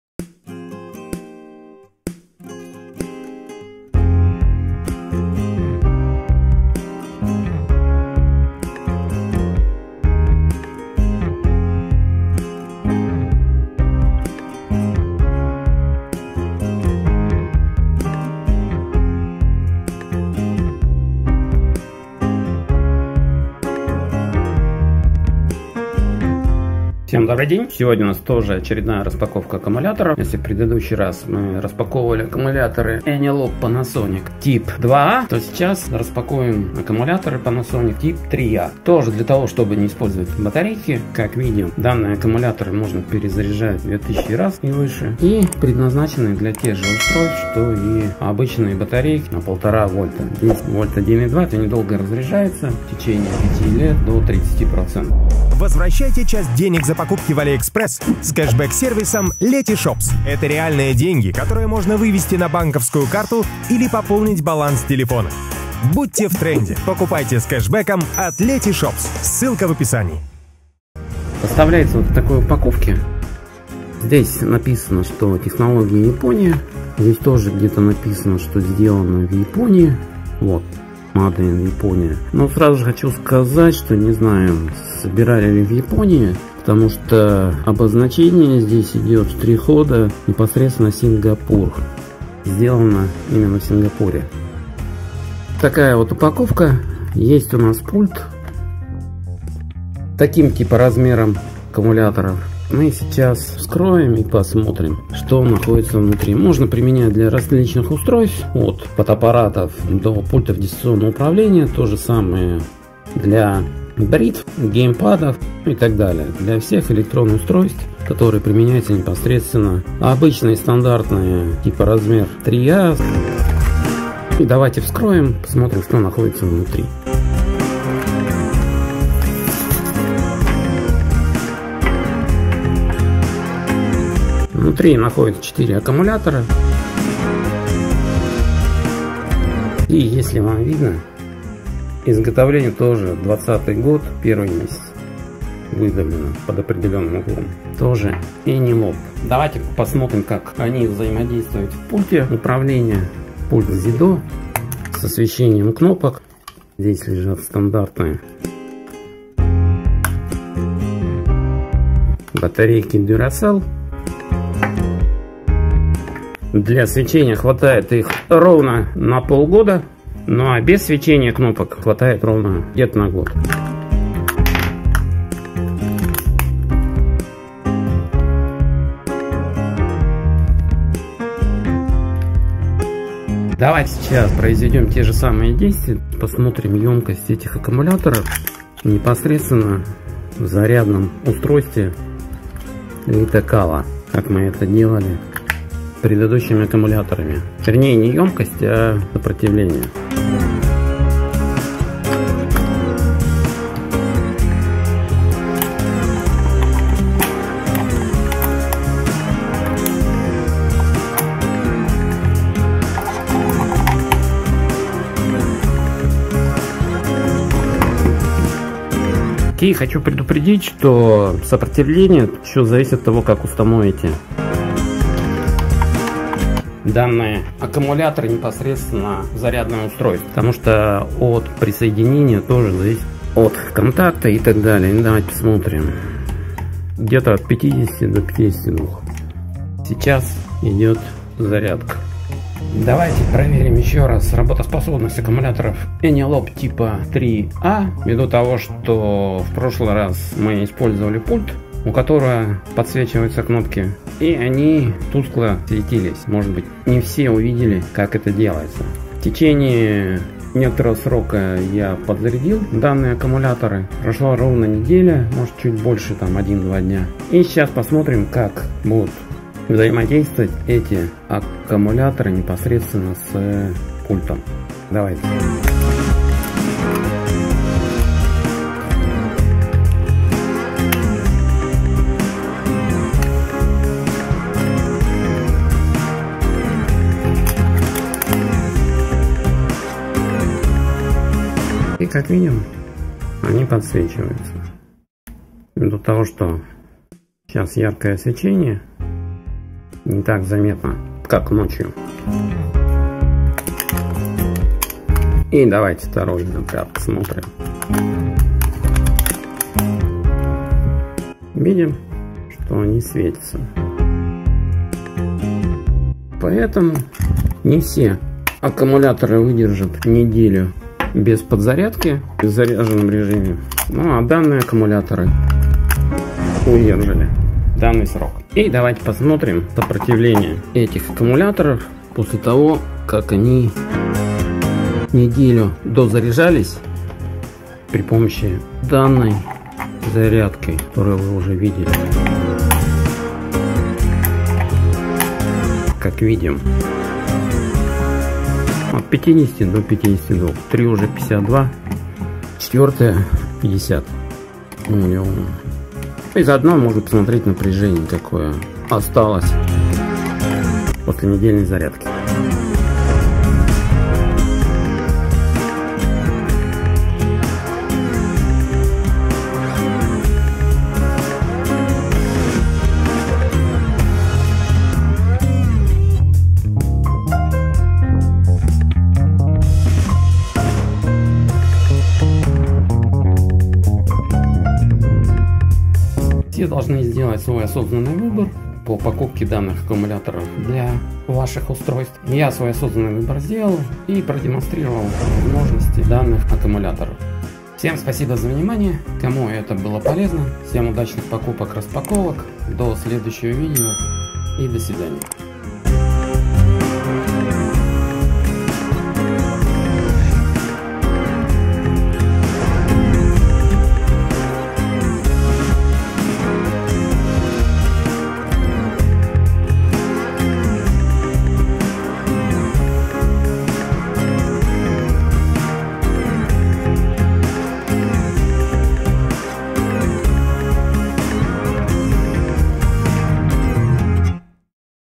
Аккумулятор Panasonic Eneloop AAA 800 мА/ч, 1,2 В, Ni-MH. Обзор!)
Оригинальный аккумулятор Panasonic Eneloop Pro AAA 800 (мин 750) мА/ч, 1,2 В, Ni-MH предварительно заряженные аккумуляторные батареи для камеры, фонарика, игрушки.

0:00 Введение
0:54 Реклама
1:11 Распаковка
2:30 Демонстрация работы 1
3:06 Тест 1
4:02 Демонстрация работы 2
5:28 Тест 2
6:25 Выводы

Снято на смартфон ASUS Zenfone Max Pro (M1) ZB602KL 3/32GB Черный ( APEXEL APL-HDAN 1.33x анаморфный объектив ), а так же отснятые вставки смартфона HTC Desire 825 Dual Sim Dark Grey ( Ulanzi 1.33x анаморфный объектив ).